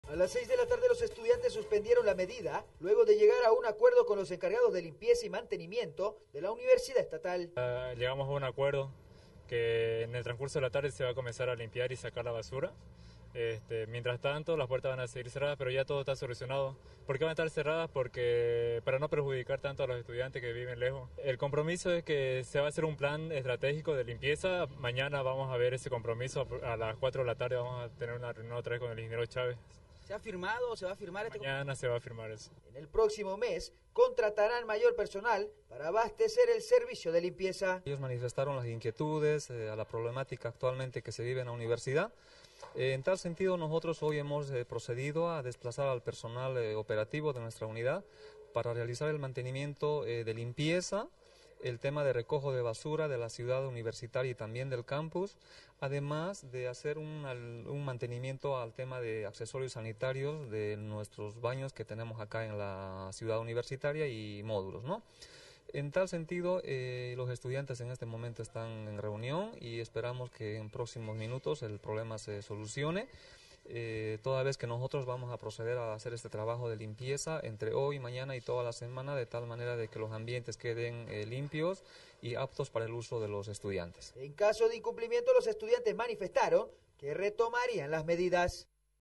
Estudiantes logran acuerdo para limpieza de la U.A.G.R.M.
Universitarios y administrativos encargados de mantenimiento de la U.A.G.R.M llegaron a un acuerdo luego de una reunión para firmar un compromiso de limpieza y mantenimiento, por lo que la medida de presión de los estudiantes fue levantada.

Levantaron la medida de presión